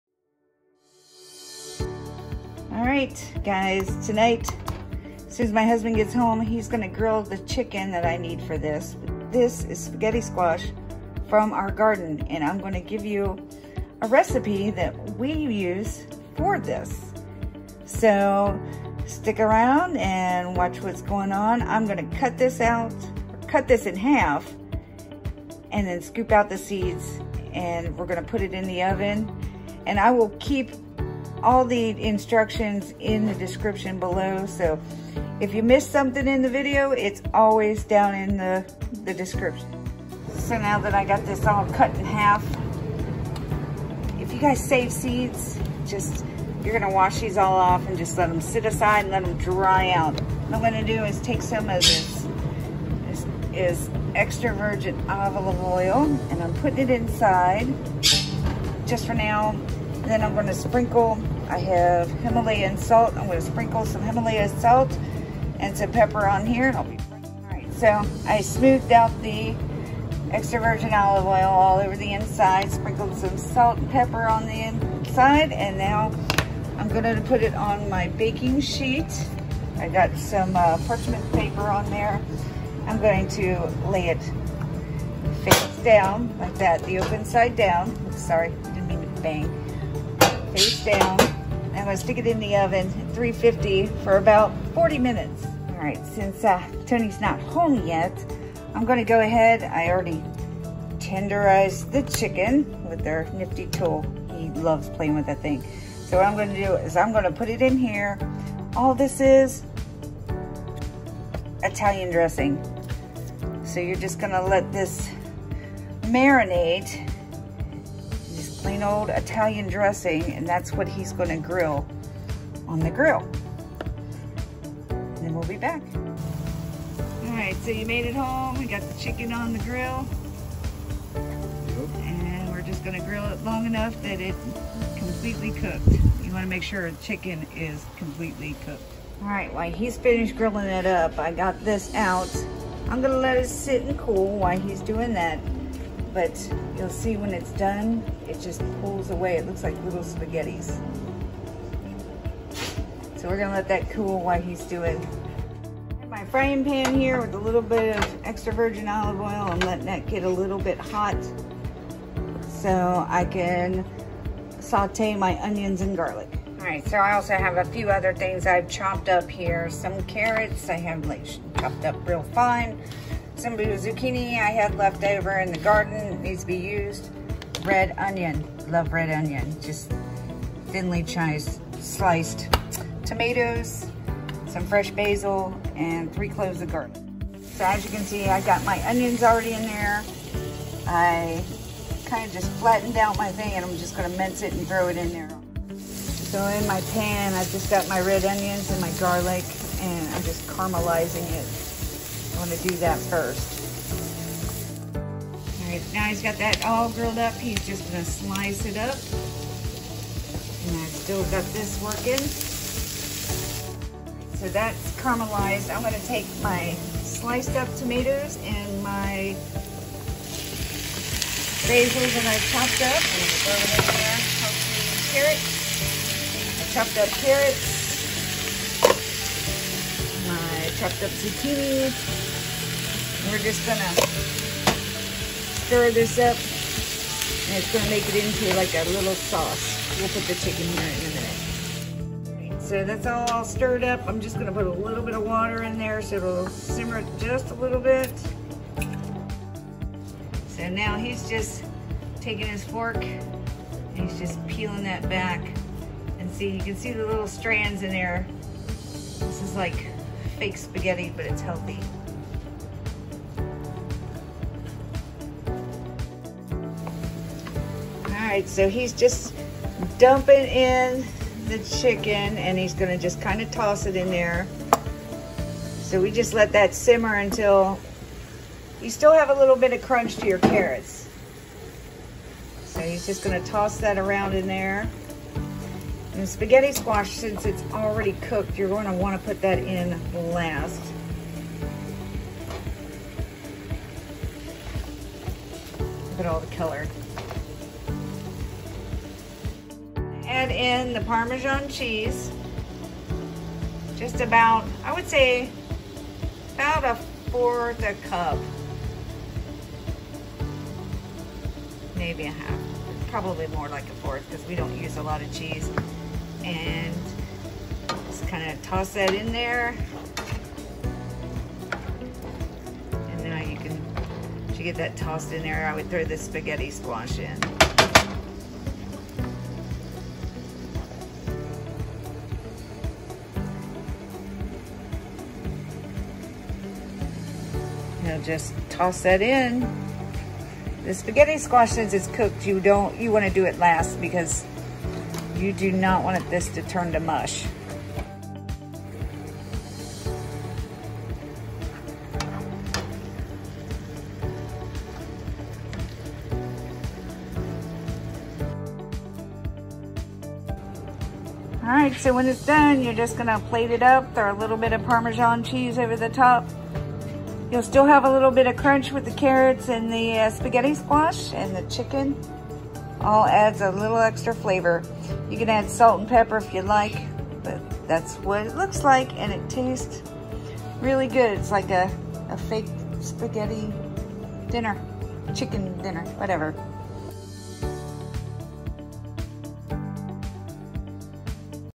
Grilled Chicken Tossed with Spaghetti Squash & Vegetables
Grilled Chicken tossed in with spaghetti squash and vegetables

Ingredients:
2 boneless chicken breast
(marinated in Italian dressing)

1 med size spaghetti squash
(Cut in half and seed removed )
Olive oil
Salt & pepper

Medium red onion sliced thin
2 large carrots chopped
1 small zucchini slice thinly
3 cloves of garlic minced
2 tomatoes chopped into small pieces.
5 leaves Fresh Basil chopped
1/4 to 1/2 cup parmesan cheese.

Coat inside of spaghetti squash with olive oil, add salt and pepper. Place facedown on baking sheet( on parchment paper).And bake at 350° for 35 to 40 minutes.
When done remove from oven and let cool. Then take a fork and scrape inside out.
After marinating chicken grill till chicken is fully cooked. Then slice into thin slices.
In large frying pan. Sauté onions and garlic till caramelized. Add in remaining vegetables. Along with a little bit of water and let’s steam on medium heat for about 10 minutes ,carrots will still be crunchy.
Toss in grilled chicken. Then Parmesan cheese. Add spaghetti squash last, making sure you don’t over cook it.

This is our Amazon link for the Meat tenderizing gadget